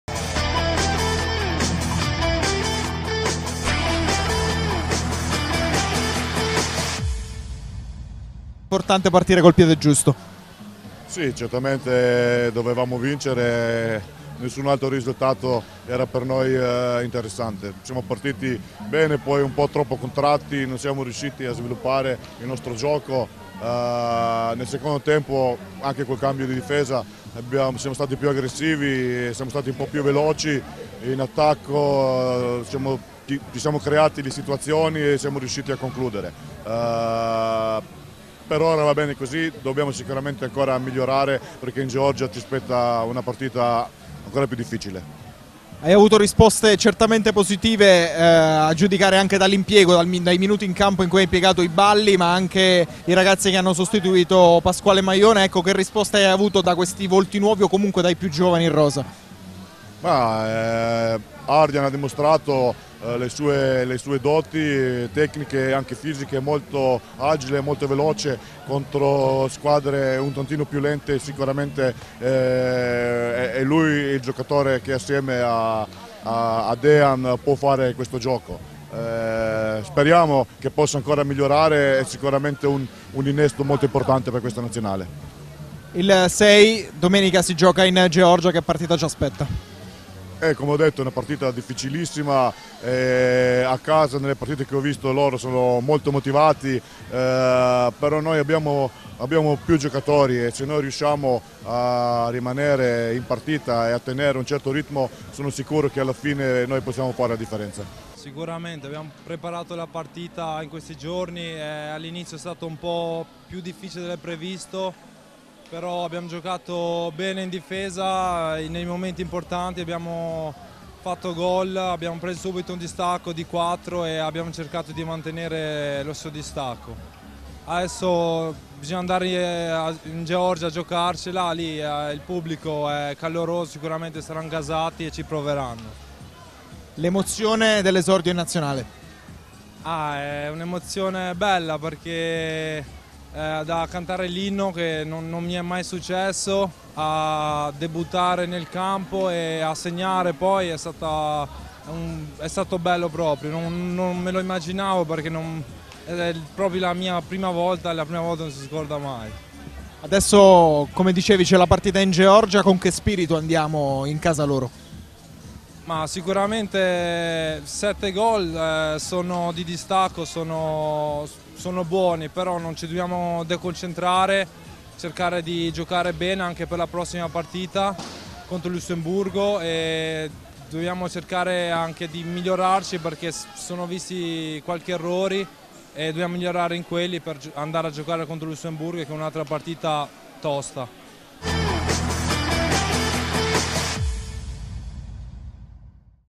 Italia - Georgia 26-19 | Post-gara
L'Italia apre con una vittoria il Gruppo C di qualificazione agli EHF EURO 2020: battuta 26-19 la Georgia al Pala Lo Bello di Siracusa. A fine gara, le dichiarazioni del tecnico Fredi Radojkovic e di Ardian Iballi, all'esordio assoluto in maglia azzurra.